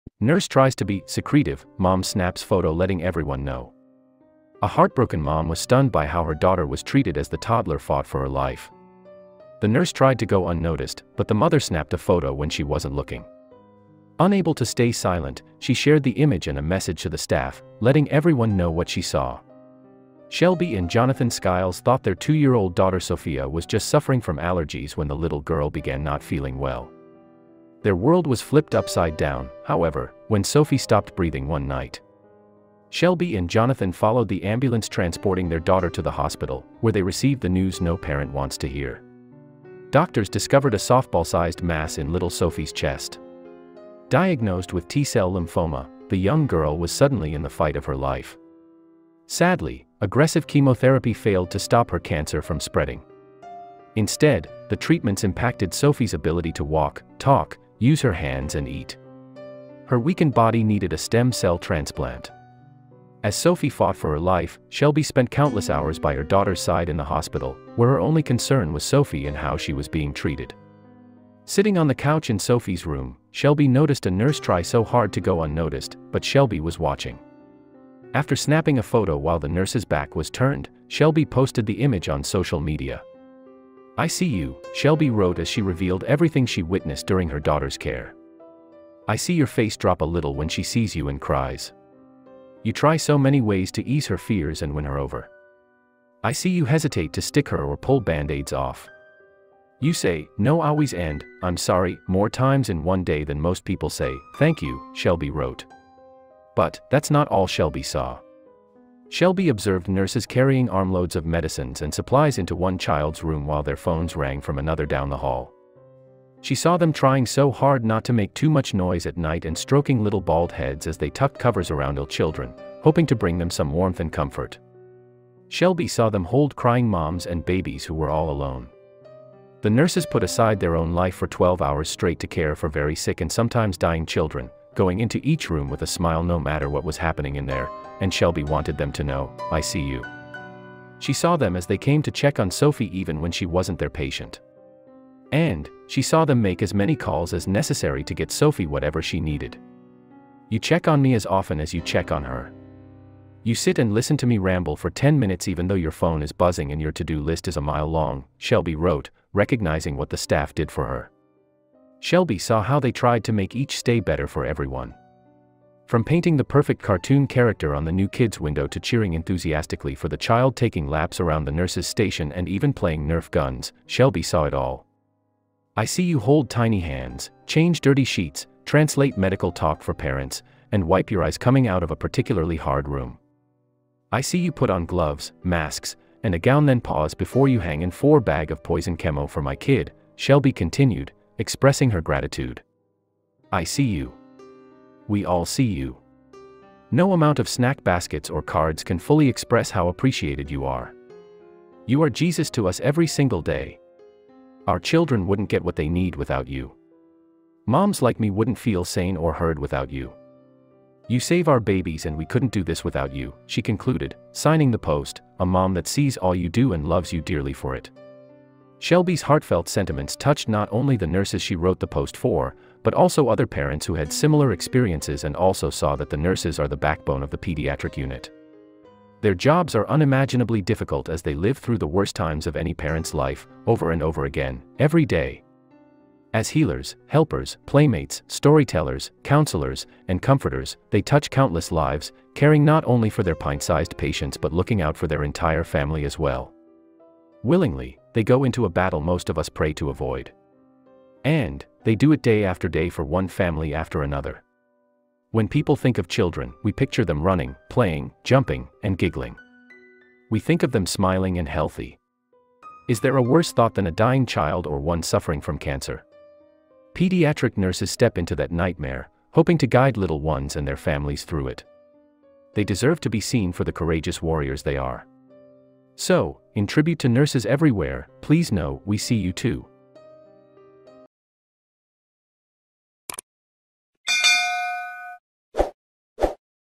Nurse Tries To Be ‘Secretive,’ Mom Snaps Photo Letting Everyone Know
A heartbroken mom was stunned by how her daughter was treated as the toddler fought for her life. The nurse tried to go unnoticed, but the mother snapped a photo when she wasn’t looking. Unable to stay silent, she shared the image and a message to the staff, letting everyone know what she saw.